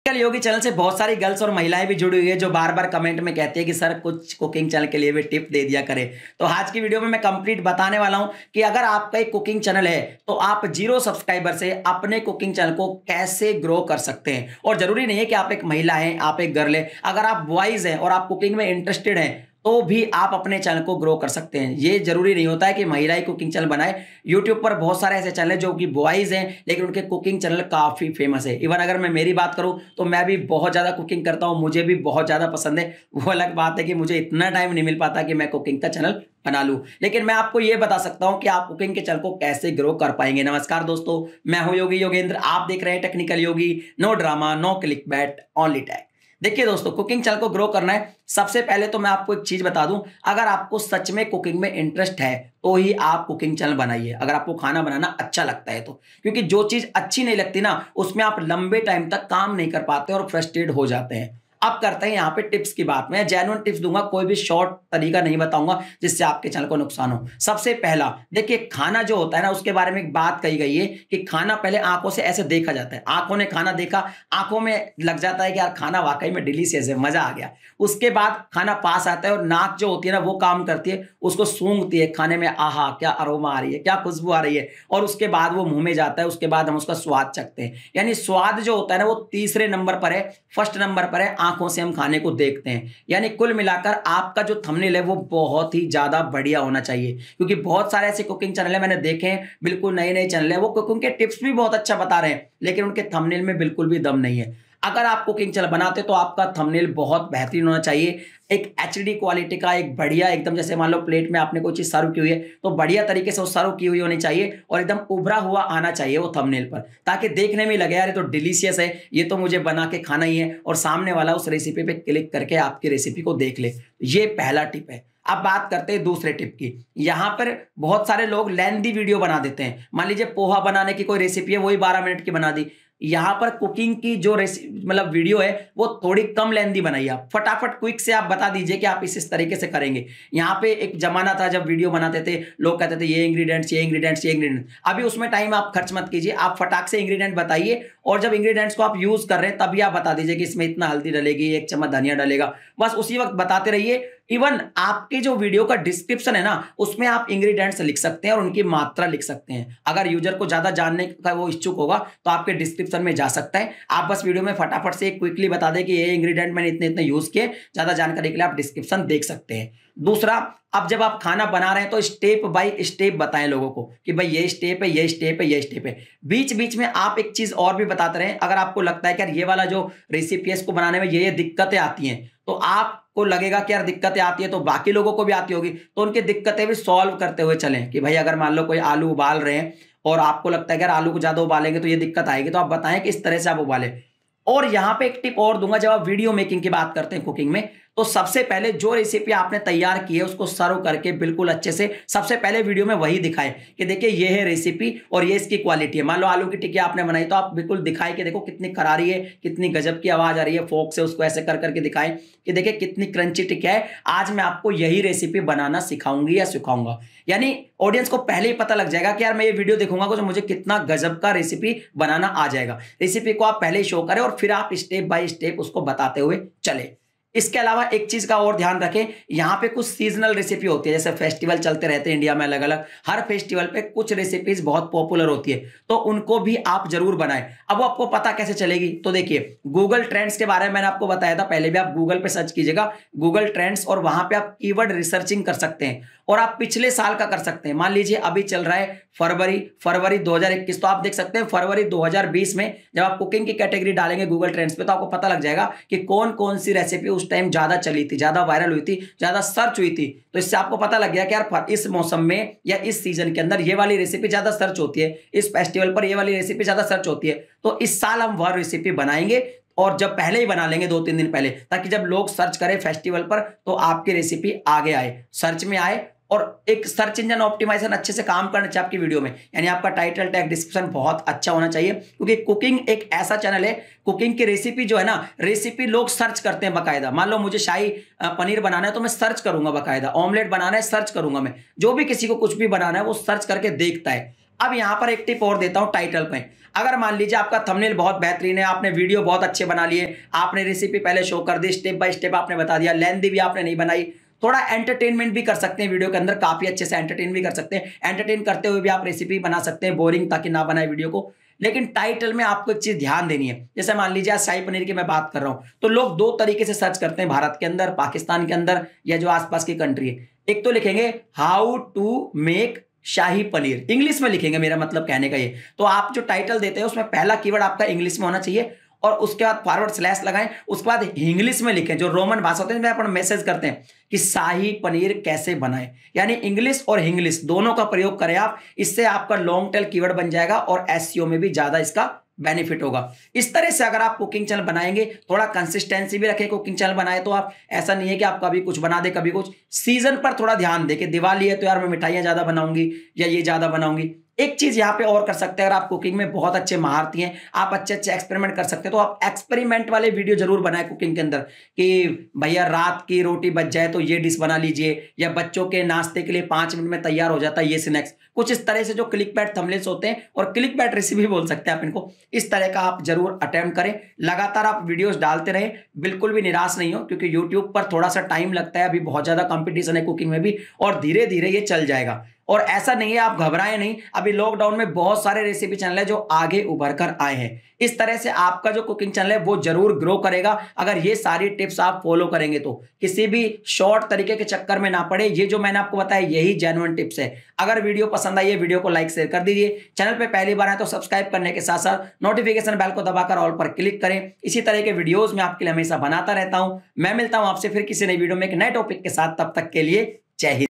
How to Grow Cooking Channel from 0 Subscribers in 2021 | Cooking Channel Grow Kaise Kare
Dosto Aaj Ki Is Video Me Maine Aapko Ye Bataya Hai Ki Aap Apne Cooking Channel Ko 2021 Me Kaise Zero Subscribers Se Grow Kar Sakte Hai Ummeed Hai Aapko Ye Video Bahut Pasand Aayegi  Thank you so much...